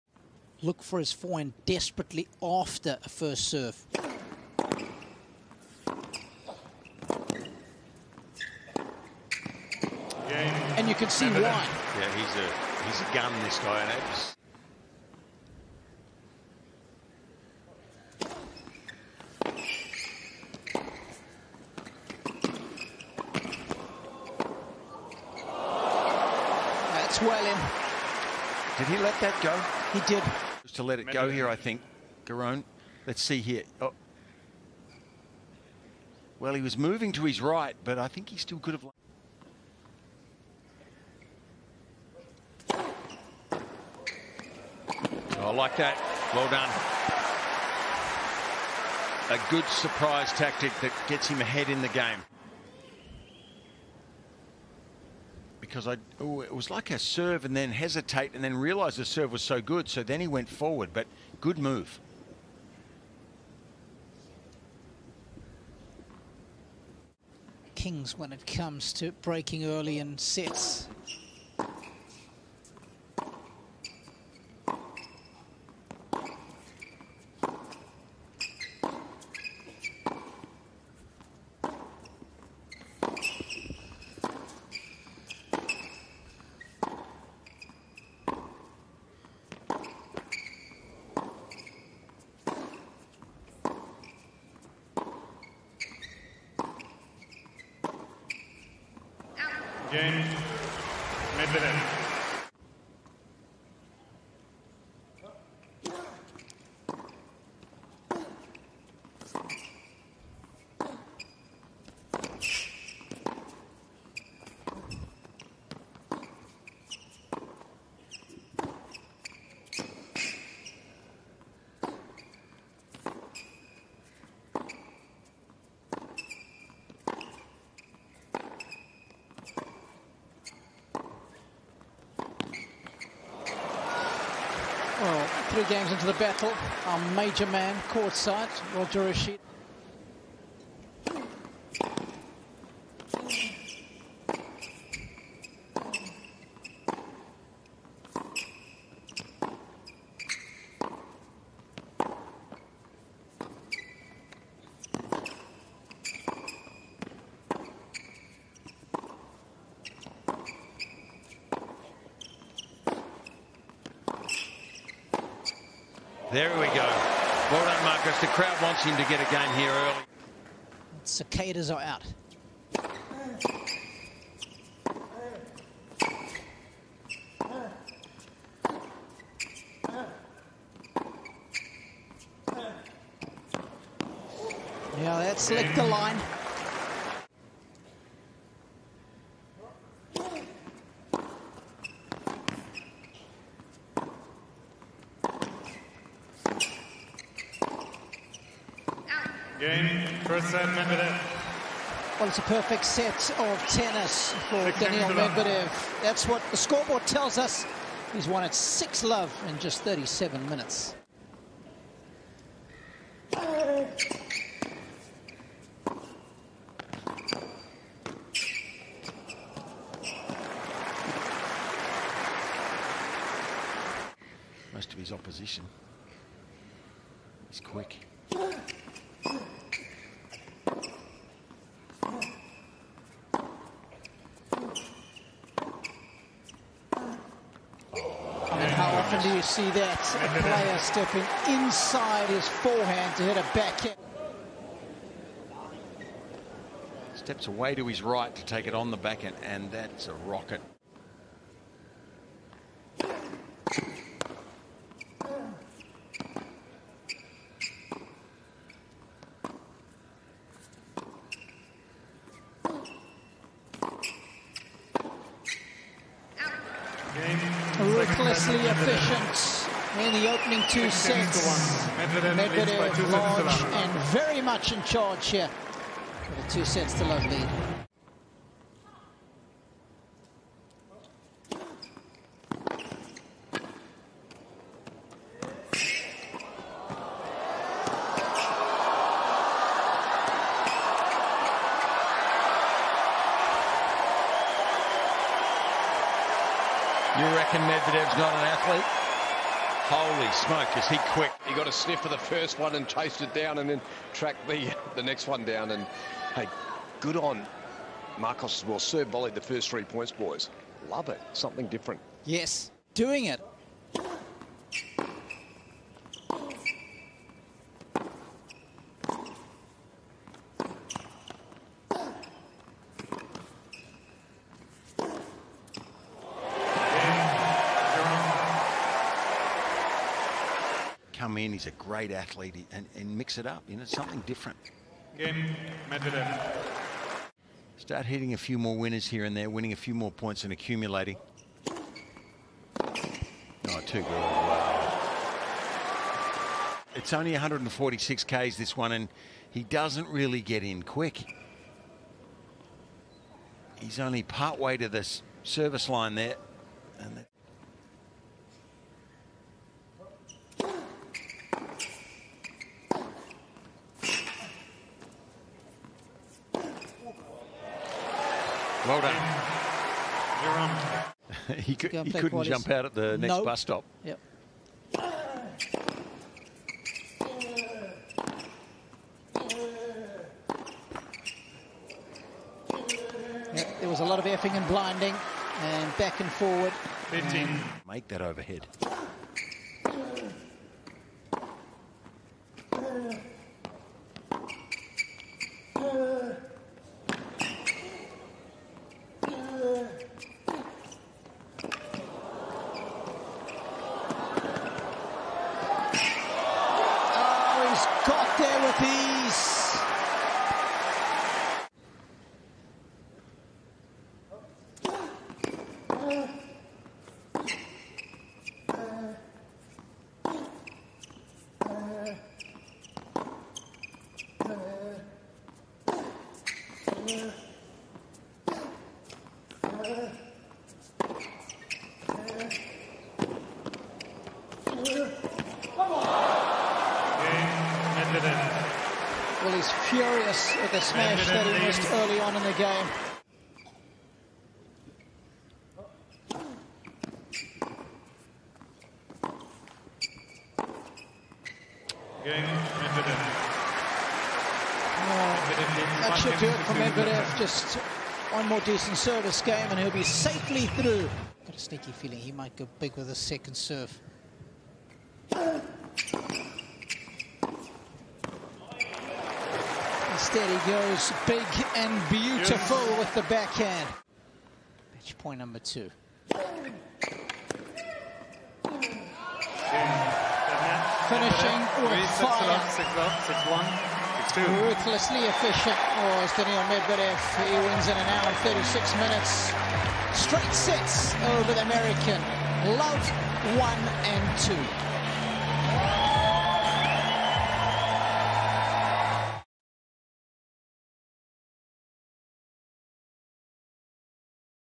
Marcos Giron v Daniil Medvedev Extended Highlights | Australian Open 2023 First Round
Watch the extended highlights of Marcos Giron v Daniil Medvedev in the first round of the Australian Open 2023.